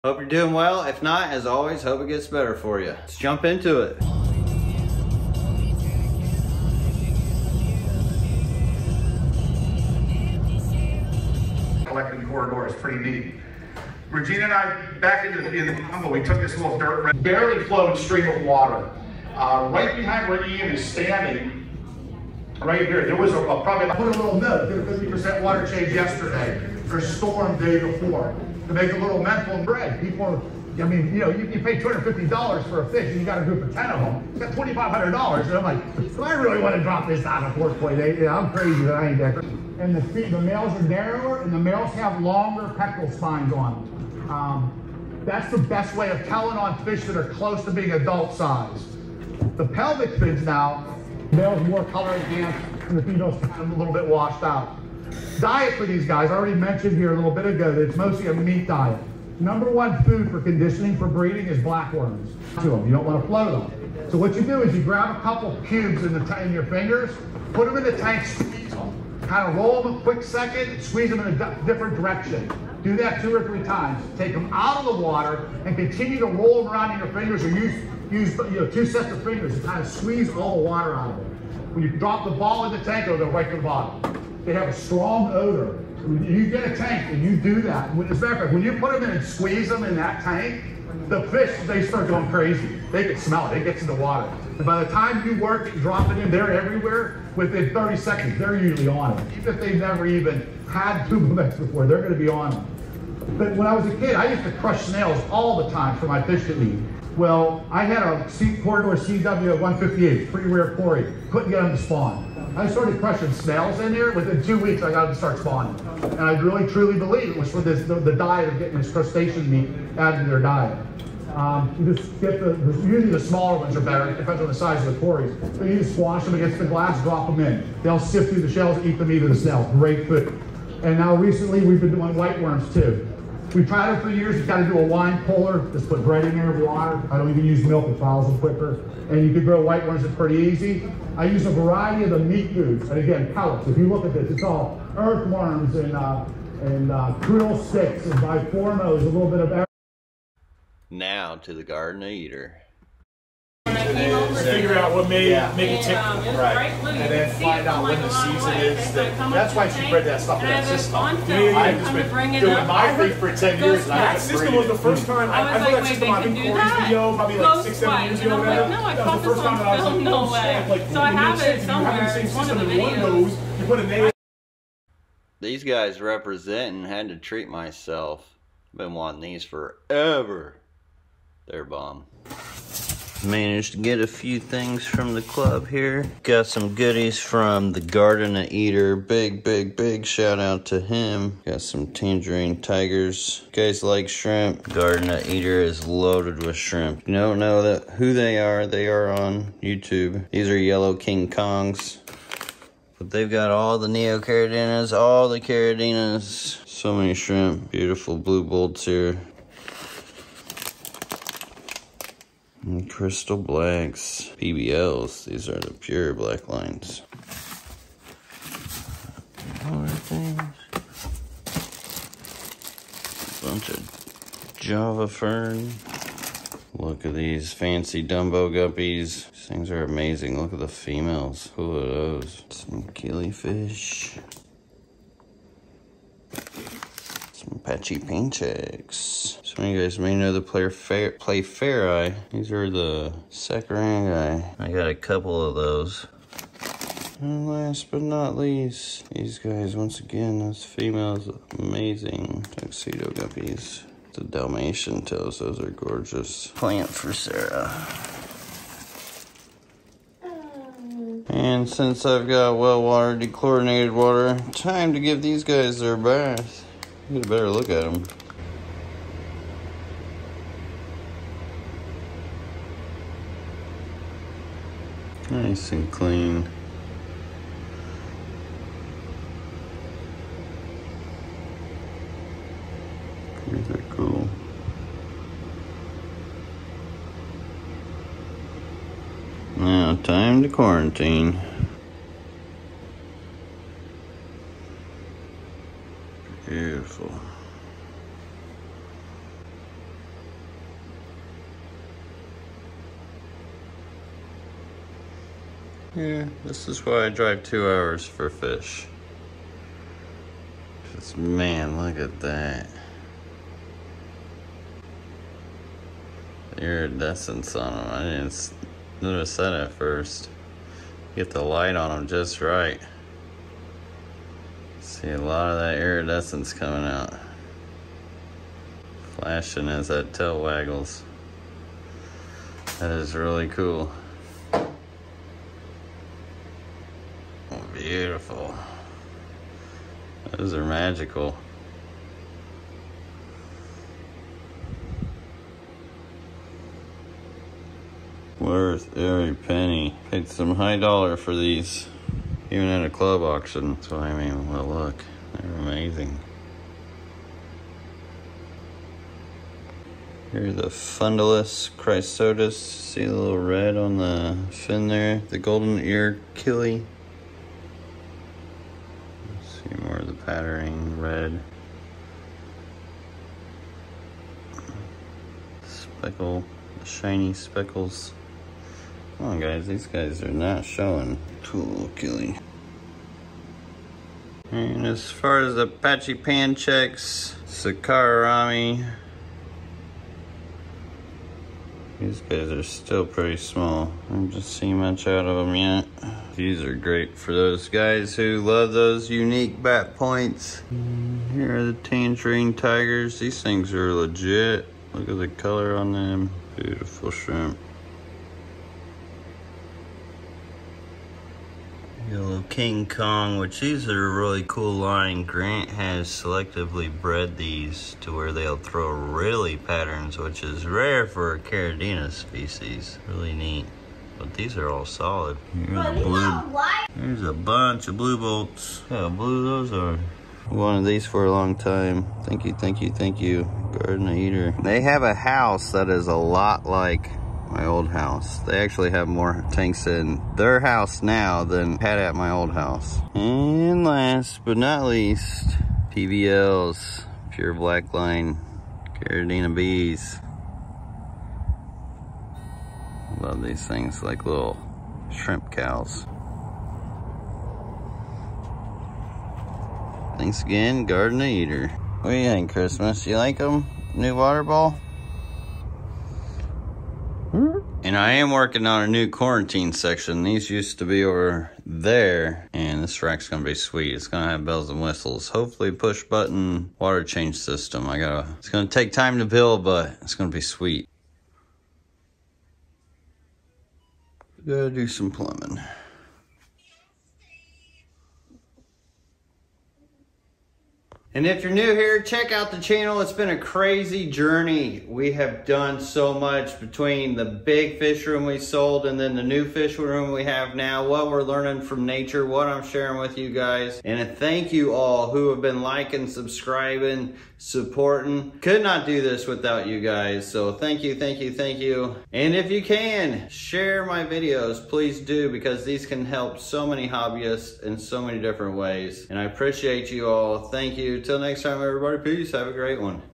Magical Guppies and Delicious Shrimp!
Wow you wont believe how awesome these guppies are I got from a club fish auction!  I didnt expect them to do what they did. Plus I got some amazing shrimp from The Garden of Eder that I have been waiting a long long time for and now have the space for them! If you love clean Homebred top quality aquatics and rarities check out the site below. :D

Want to support or represent, snag up a LRB Aquatics Mad Scientist Shirt here is the

LRB